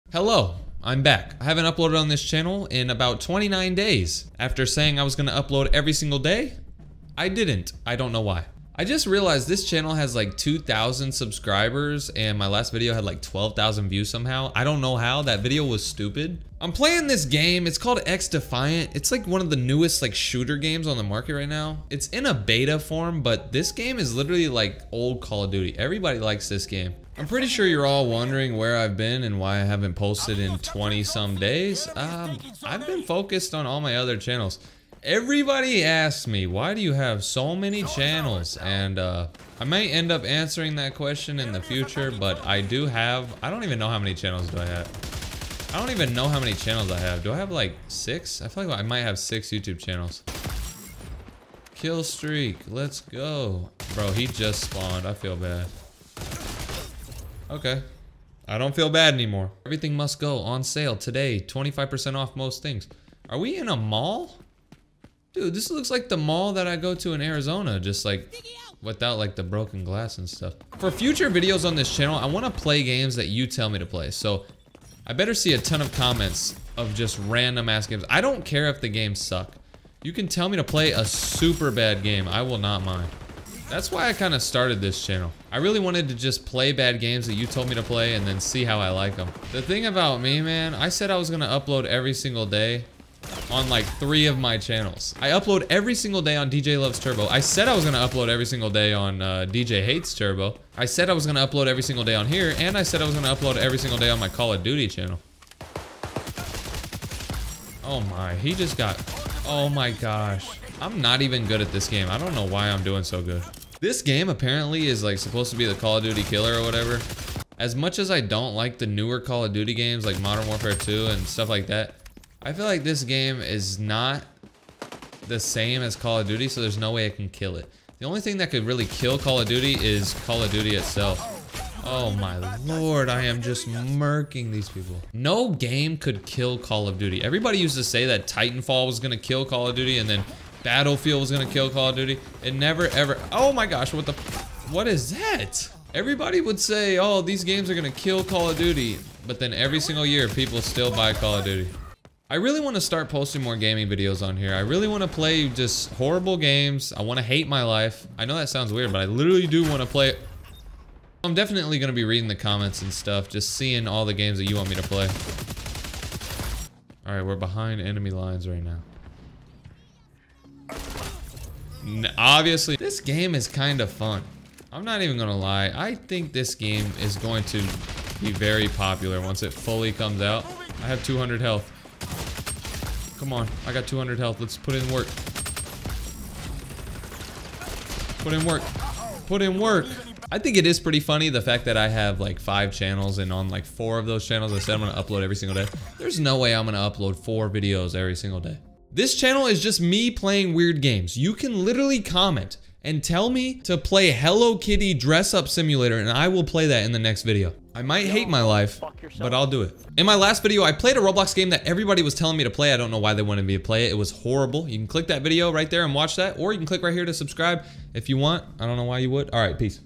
I Haven't Uploaded in 29 Days..
I said I was going to upload daily, and then didn’t upload for a month 😂 sowwy